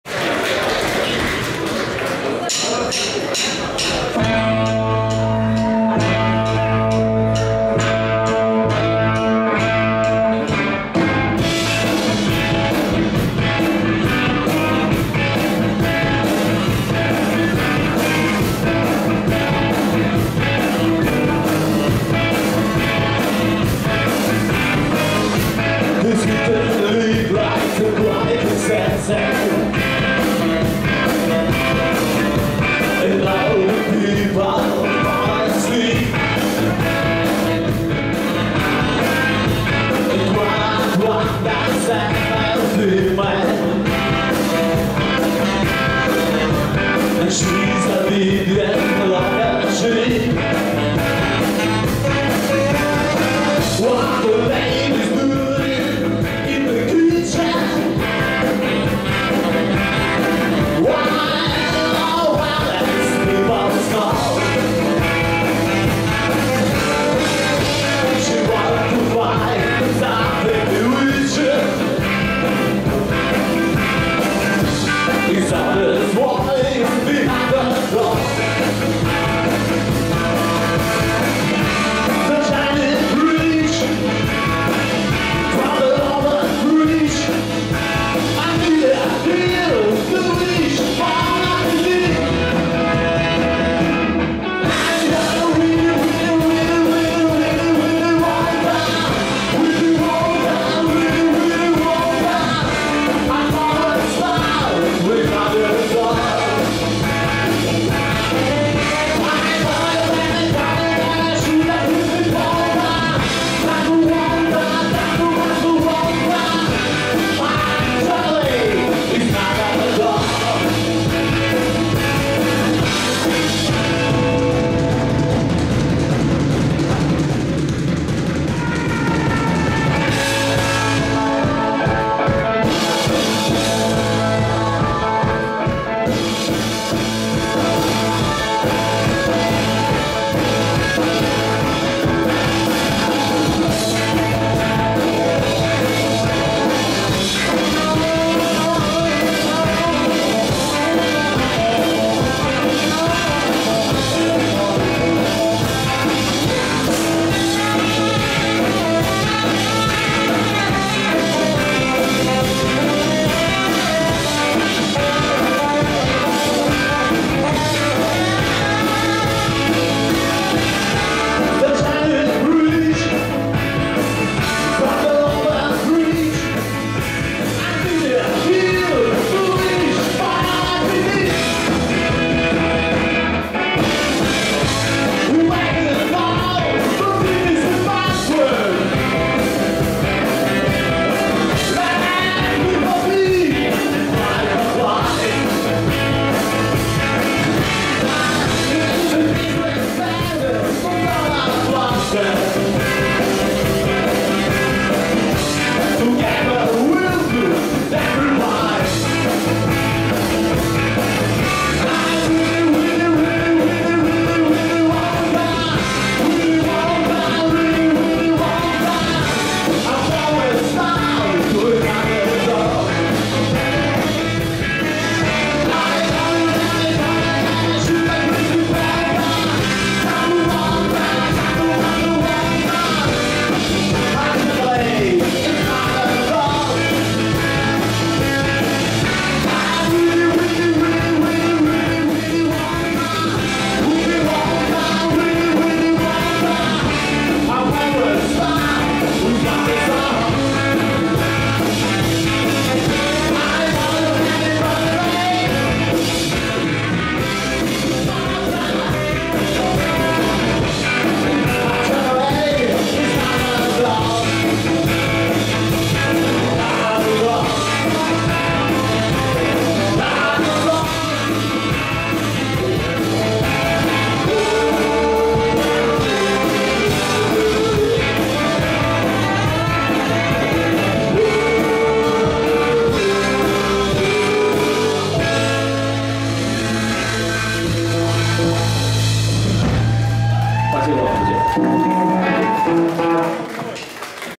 The Bad Band - (?) @ Brooklyn 12.04.2016
Празднование Дня Рождения звукозаписывающей студии Pitone Sound